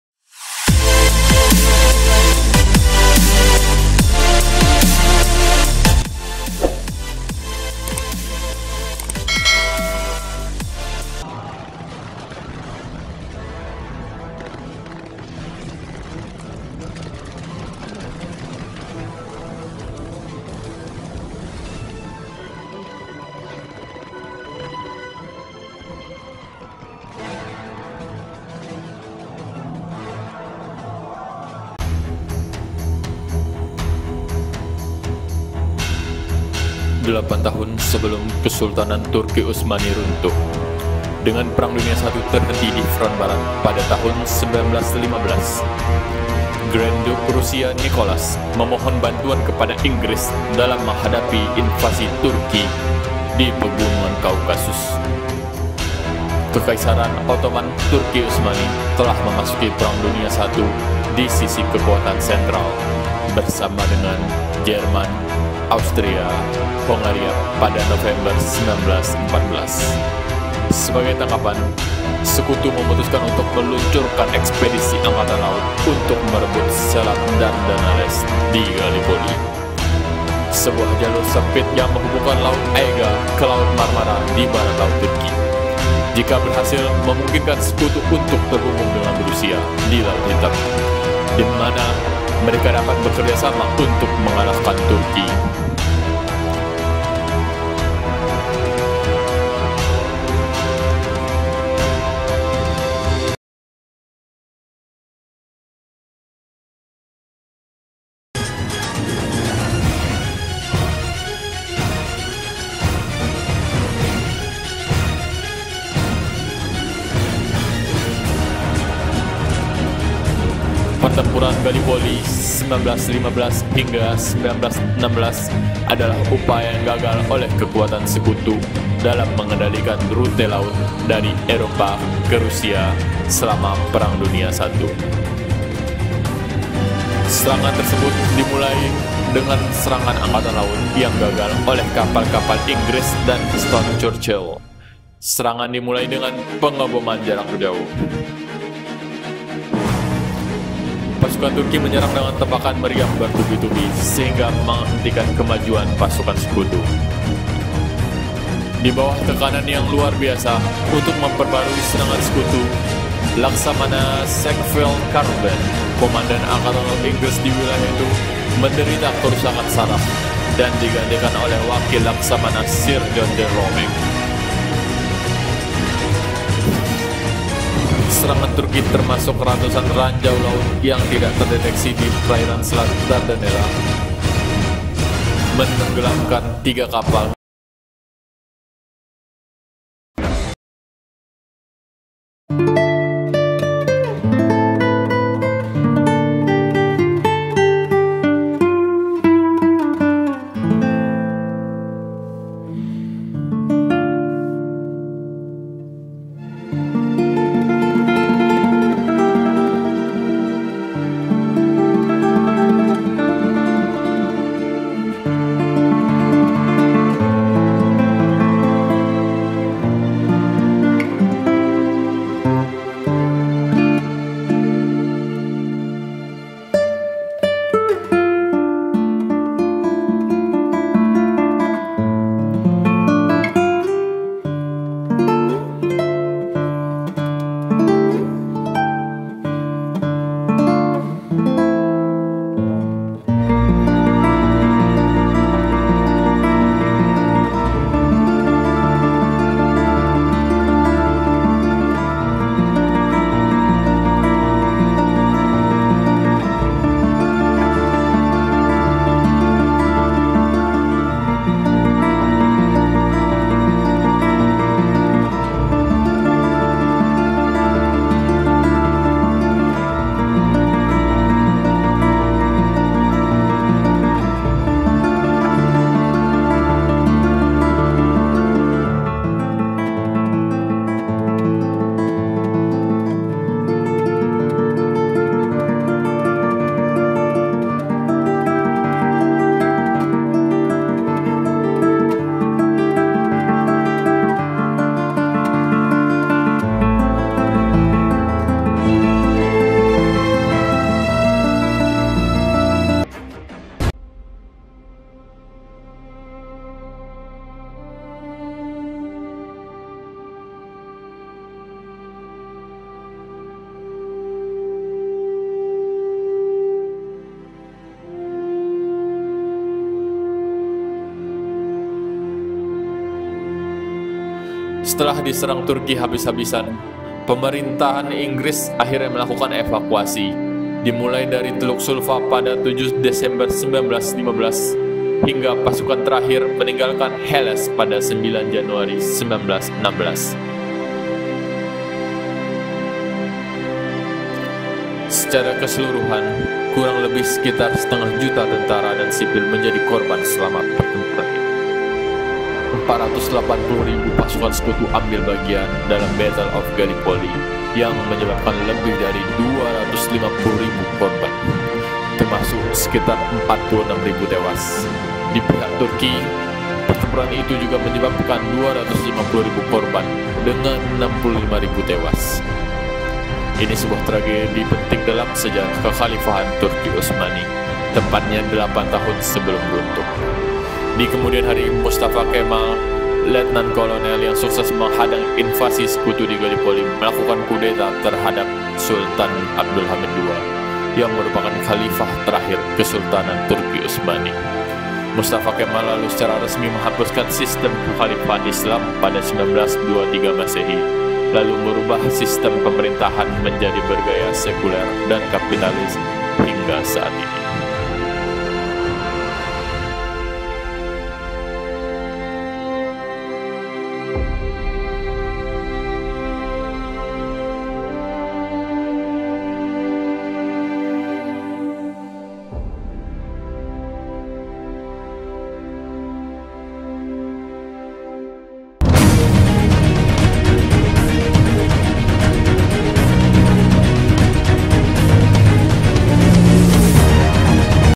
PERANG GALLIPOLI 1915 : EPIC COMEBACK! TURKI OTTOMAN VS 4 NEGARA BESAR (PD 1)
PERANG GALLIPOLI 1915 : EPIC! TURKI OTTOMAN VS 4 NEGARA BESAR (Perang Dunia 1)  8 tahun sebelum Kesultanan Turki Ottoman runtuh.
Dengan Perang Dunia 1 terhenti di Front Barat pada tahun 1915. Grand Duke Rusia Nicholas memohon bantuan kepada Inggris dalam menghadapi invasi Turki di Pegunungan Kaukasus. Kesultanan Ottoman (Turki Utsmani) telah memasuki Perang Dunia 1 di sisi Kekuatan Sentral,  bersama dengan Jerman, dan Austria-Hongaria, pada November 1914).
Sebagai tanggapan, Sekutu memutuskan untuk meluncurkan ekspedisi angkatan laut untuk merebut Selat Dardanelles,Gallipoli —sebuah jalur laut sempit yang menghubungkan Laut Aegea ke Laut Marmara di barat laut Turki. Jika berhasil, memungkinkan Sekutu untuk terhubung dengan Rusia di Laut Hitam, di mana mereka dapat bekerja sama untuk mengalahkan Turki.
Pertempuran Gallipoli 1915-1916 adalah upaya yang gagal oleh Kekuatan Sekutu untuk mengendalikan rute laut dari Eropa ke Rusia selama Perang Dunia 1.
Serangan tersebut dimulai dengan serangan angkatan laut yang gagal oleh kapal-kapal Inggris dan Prancis di Selat Dardanelles pada 18 Maret 1915. Penyerangan ini dipelopori oleh First Lord of the Admiralty Winston Churchill, serangan dimulai dengan pengeboman jarak jauh.
Pasukan Turki menyerang dengan tembakan meriam bertubi-tubi, sehingga menghentikan kemajuan pasukan sekutu.
Di bawah tekanan yang luar biasa, untuk memperbarui serangan sekutu, Laksamana Sackville Carden—komandan Angkatan Laut Inggris di wilayah itu—menderita kerusakan saraf dan digantikan oleh Wakil Laksamana Sir John de Robeck.
Serangan Turki—termasuk ratusan ranjau laut yang tidak terdeteksi di perairan selat Dardanella—menenggelamkan tiga kapal dan merusak tiga kapal lainnya.
Setelah serangan angkatan laut yang gagal.
Berlanjut dengan invasi darat skala besar di Semenanjung Gallipoli pada tanggal 25 April 1915, yang melibatkan pasukan Inggris, Prancis, resimen Gurkha Inggris (India Britania) serta Australia, dan Selandia Baru yang lebih dikenal Australian and New Zealand Army Corps (ANZAC),
Menanggapi upaya sekutu untuk melakukan invasi skala besar.
Turki meningkatkan pertahanan mereka di bawah komando jenderal Jerman Otto Liman von Sanders dan Letnah Kolonel Mustafa Kemal yang dikemudian menjadi Bapak Turki Modern. Mereka mulai menempatkan pasukan Ottoman di sepanjang pantai.
Pasukan Turki yang dipimpin Mustafa Kemal sukses menghancurkan pendaratan pasukan Australia dan Selandia Baru.
Pasukan Inggris juga menghadapi perlawanan sengit saat mendarat di Semenanjung Helles. Bahkan korban yang jatuh di pihak Inggris mencapai dua pertiga jumlah pasukan.
Selama tiga bulan berikutnya, pasukan Sekutu hanya membuat sedikit kemajuan yang dibayar dengan jumlah korban yang amat besar.
Untuk memecahkan kebuntuan, Inggris mendaratkan pasukan baru di Teluk Suva pada 6 Agustus 1915. Namun, Inggris juga mengalami kegagalan meski saat mendarat tak mendapat banyak perlawanan.
Setelah  diserang Turki habis-habisan. Pemerintah Inggris akhirnya melakukan  evakuasi dimulai dari Teluk Sulva pada 7 Desember 1915; pasukan terakhir meninggalkan Helles pada 9 Januari 1916.
Secara keseluruhan kurang lebih sekitar  setengah juta tentara dan sipil menjadi korban selama pertempuran ini. 480.000 pasukan Sekutu ambil bagian dalam Battle of Gallipoli, yang menyebabkan lebih dari 250.000 korban, termasuk sekitar 46.000 tewas. Di pihak Turki, pertempuran itu juga menyebabkan 250.000 korban, dengan 65.000 tewas.  Dikemudian hari peristiwa ini dikenal sebagai bagian dari disaster of war.
Inilah sebuah tragedi penting dalam sejarah Kekhalifahan Turki Utsmani. Tepatnya 8 tahun sebelum runtuh.
Kejadian ini menimbulkan luka dan trauma yang mendalam bagi pihak sekutu.
Dan menjadi momen penting bagi Turki ,untuk meningkatkan moralitas tentara dan rakyat.
Dimana saat itu, Turki Utsmani telah lemah, sehingga dijuluki “Sick Man” oleh negara-negara Eropa.
Di Kemudian Hari , Mustafa Kemal , Letnan Kolonel yang sukses menghadang invasi sekutu di Gallipoli melakukan kudeta terhadap Sultan Abdul Hamid II, yang merupakan Khalifah terakhir Kesultanan Turki Utsmani.
Mustafa Kemal lalu secara resmi menghapuskan sistem kekhalifahan Islam pada 1923 M, lalu merubah sistem pemerintahan menjadi bergaya sekuler dan kapitalis hingga saat ini.